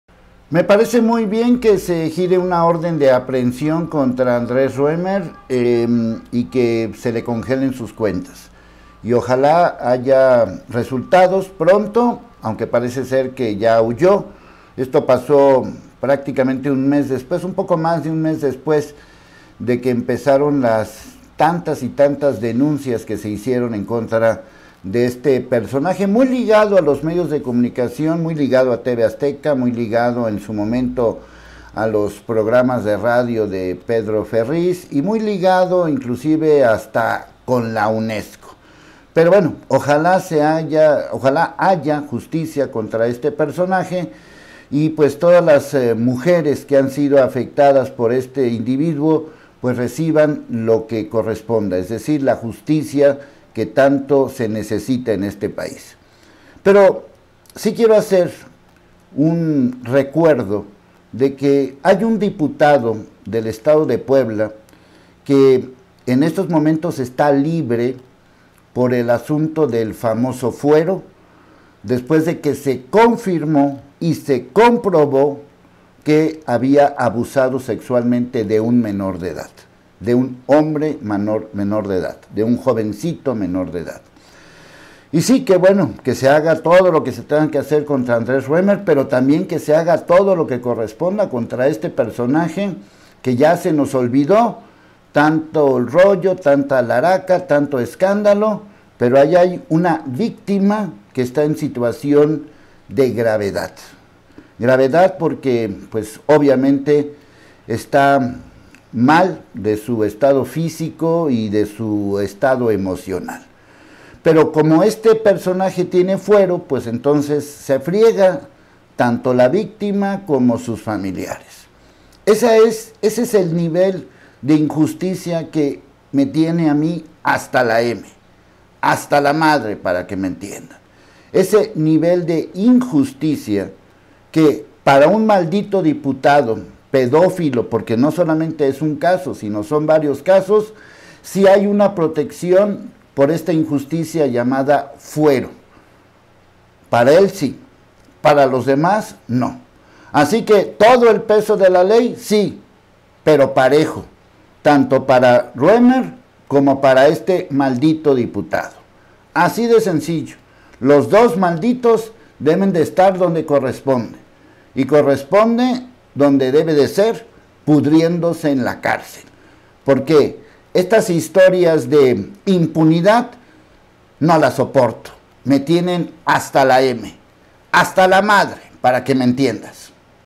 La justicia debe ser pareja
Bien vayan contra Reomer pero mal que dejen impune a diputado de Morena

Apoya el periodismo independiente.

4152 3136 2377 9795